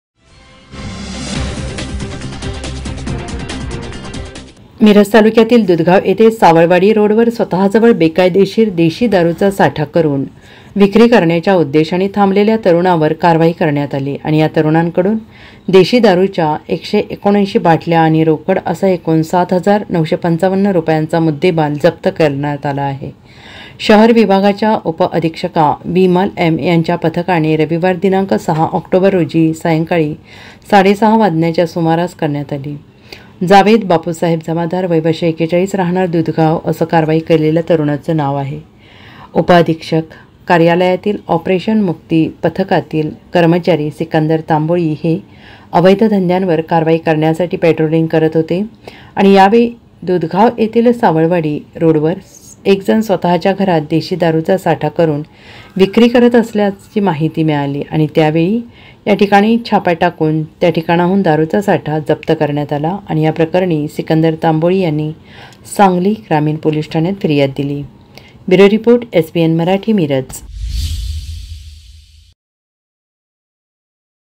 SANGLI | बेकायदेशीर देशी दारूचा साठा करून विक्री करण्याच्या उद्देशाने थांबलेल्या तरुणावर कारवाई
दुधगाव येथे सावळवाडी रोडवर स्वतः जवळ बेकायदेशीर देशी दारूचा साठा करून विक्री करण्याच्या उद्देशाने थांबलेल्या तरुणावर कारवाई